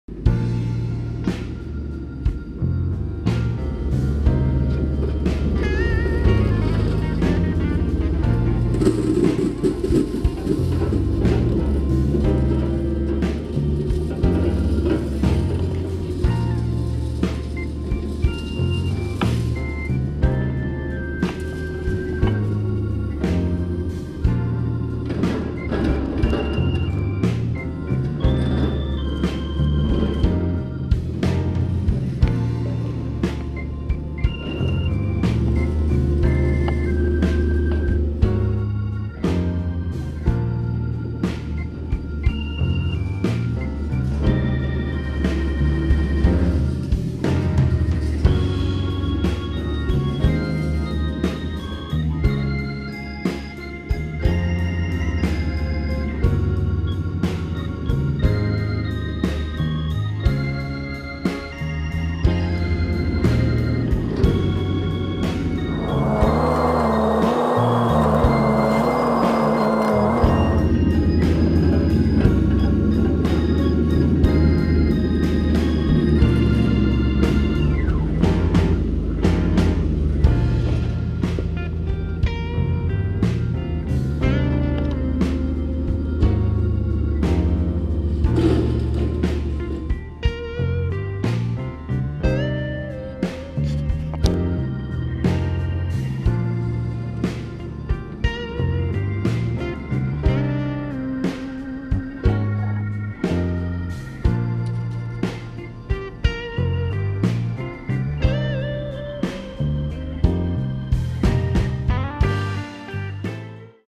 Linja- autoasema 20.11. 2019. Westmedia Oy. Rauma
Linja-autoasema alkaa olla kasassa. Kuvaus & edit. Westmadia Oy. Rauma. kuvaa jo vuodesta 1988.